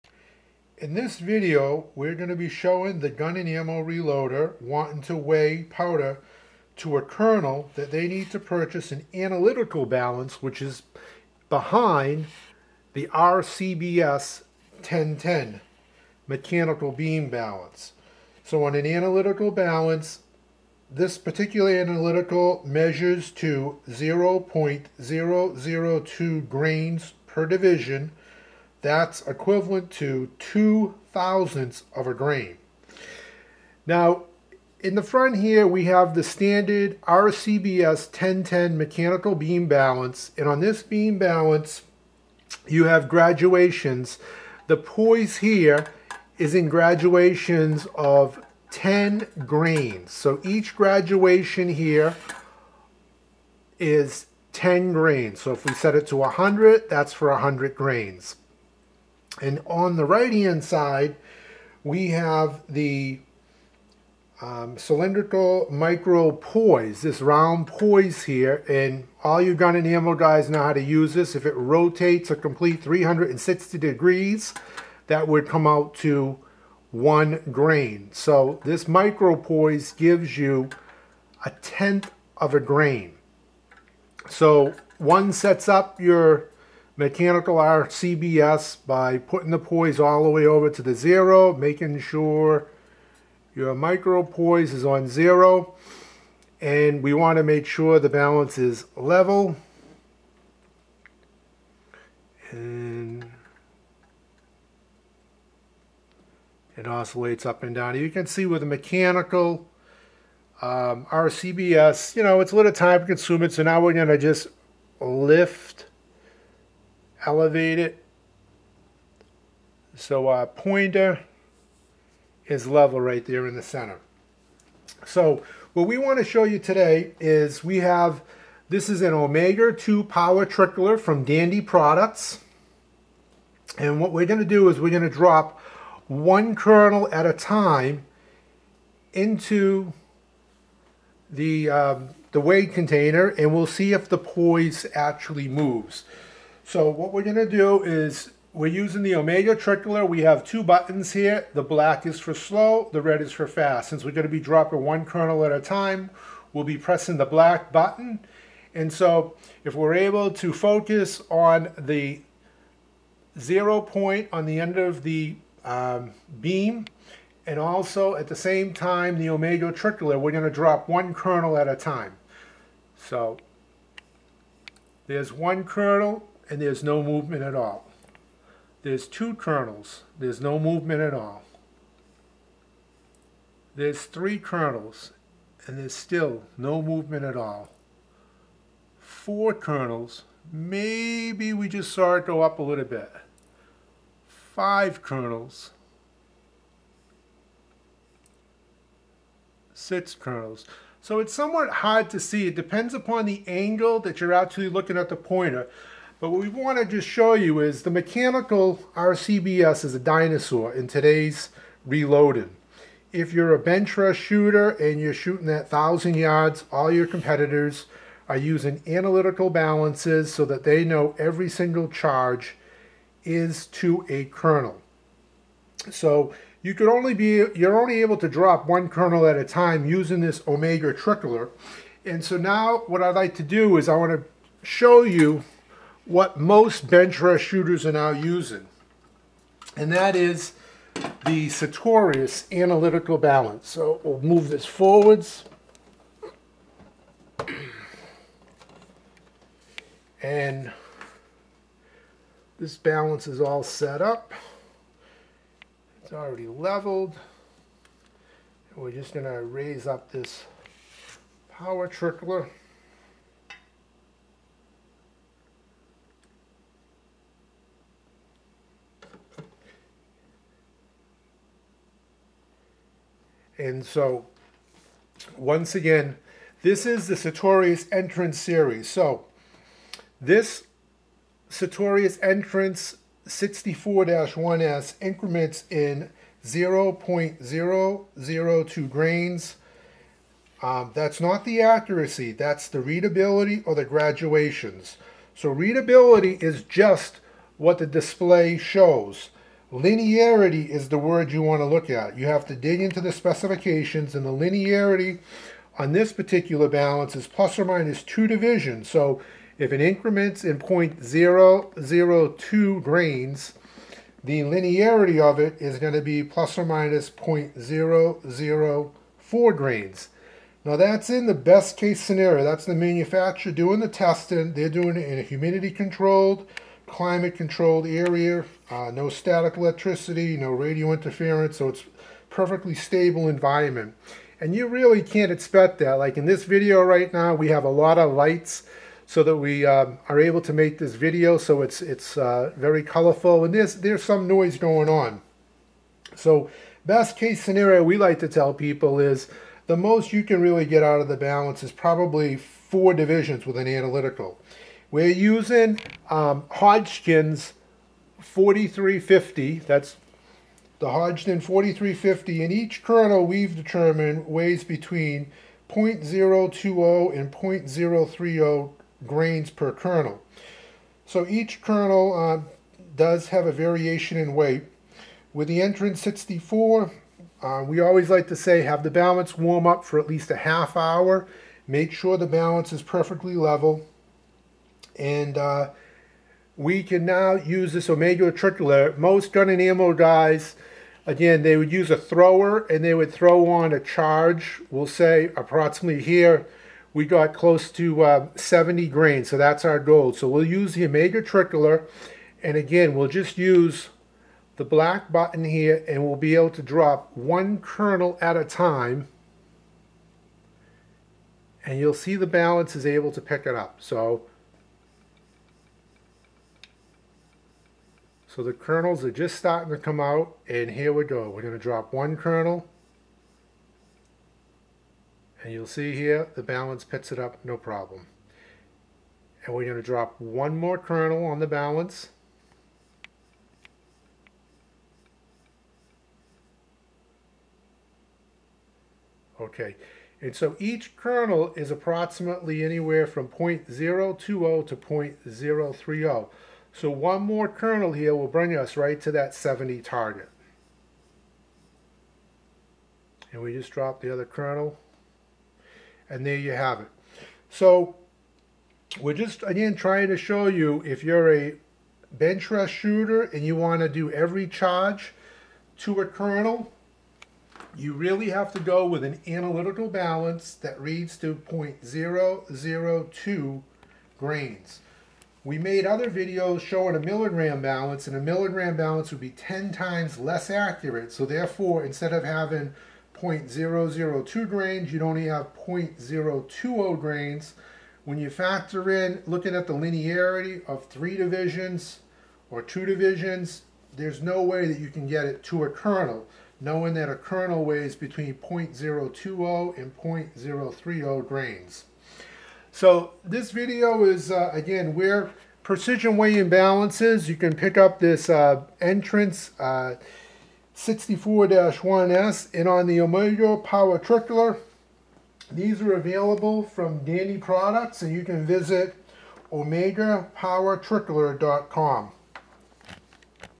Ammo reloaders weighing powder to a kernel RCBS10-10 vs Analytical Balance
In this demonstration we are going to show ammunition reloaders wanting to weigh powder to a kernel they need to purchase an analytical balance that increments in 0.002 grain divisions/graduations. That is two thousandth of a grain.

Now just because a balance offers readability to 0.002 grain divisions/graduations does not mean it is accurate to +/- 0.002 grain. Readability is just what the balance's displays resolution. One needs to dig into the manufacturer's specification  and look for the word "linearity" which is the accuracy of the balance. Depending on the manufacturer the linearity is +/- 2d, 3d or some low cost Chinese made balances it could be 4d or 5d. In this video we will be using the Sartorius Entris64-1S and the linearity is stated as +/- 2 divisions. The Entris increments in 0.002 grain divisions so the linearity is +/- 0.004 grains. These manufacturer specs are performed by the manufacturer and done in controlled environments (temperature & humidity controlled, no static electricity issues, no radio frequency interference, perfectly stable air so there are no drafts). You have to have faith the manufacturer is honest in their testing and we have seen over and over again the no name brands stated specs do not correspond to the performance of the balance. Since testing is done in ideal environments you really can't expect the same performance and let's just say if the manufacturer states +/- 2d with your environment you'll get +/- 4d.

A kernel of Hodgon H4350 weighs between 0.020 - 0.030 grains per kernel. Each kernel weight varies. So for the Sartorius Entris64-1S I like to say when you have your balance level, balance warmed up for at least 30 minutes, in a stable environment realistically you'll expect to have accuracy to 0.008 grains (0.002gn x 4d = +/-0.008gn). Now if you purchased a milligram balance the readability and linearity would be 10 times less of an analytical balance so the best performance you could expect is 0.08 grains. If a kernel weighs between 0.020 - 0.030 grains per kernel there is no way a milligram balance can deliver the consistent weighing results the competition benchrest shooting is looking for which is knowing each round of ammunition has the same amount of powder. Having ammo with an extra kernel or short a kernel will result in the bullet being high or low of the target's bullseye .

The RCBS10-10 mechanical beam scale finest graduation is 0.1 grains. The large poise increments in 10 grain divisions, the cylindrical micrometer poise has graduation to 0.1 grains and uses a gauge that increments in 1 grain divisions. One 360 degree rotation of the micrometer poise yield 1 grain.

If we say the average weight of a kernel of H4350 is 0.025 grains then 4 kernels weigh 0.1 grains, which is the best readability the RCBS 10-10 offers. It is not the accuracy since you are moving parts, friction points and the agate bearing the fulcrum pivots on wears. We'll use the Omega 2 Speed Powder Trickler from Daddy Products available and we will dispense one kernel at a time to see how many kernels it takes to move the beam of the 10-10. Now watch closely as I drop one kernel while also looking at the beam pointer. I am going to press the BLACK button on the Omega Trickler and drop 1, 2, 3, 4 5 kernels. You'll notice it takes 4 to 5 kernels to see the beam even move. If you are doing any competition shooting and weighing powder with the 10-10 and other shooters are using an analytical balance you can see you are at a great disadvantage.

Now we will show you how to achieve weighing powder so you know each round is weighed to the kernel.

For many years those competition shooters wanting to weigh powder to the kernel purchased the Sartorius GD503 from Precision Weighing Balances The GD503 was discontinued in 2012 by Sartorius. Once the GD503 were sold out Precision Weighing balances alternative to the GD503 was the AND HR100a and finally in May of 2014 Sartorius re-entered the market with an affordable analytical balance called the Entris Series. The Entris64-1S weighs up to 926 grains & increments in 0.002 grain divisions.

We will now show you how the Entris64-1S can weigh to the kernel using the  Omega 2 Speed Powder Trickler.

In closing if you need any balance visit either of Precision Weighing Balances' website at either where you can buy the Entris64-1S. Omega 2 Speed Powder Trickler from Dandy Products available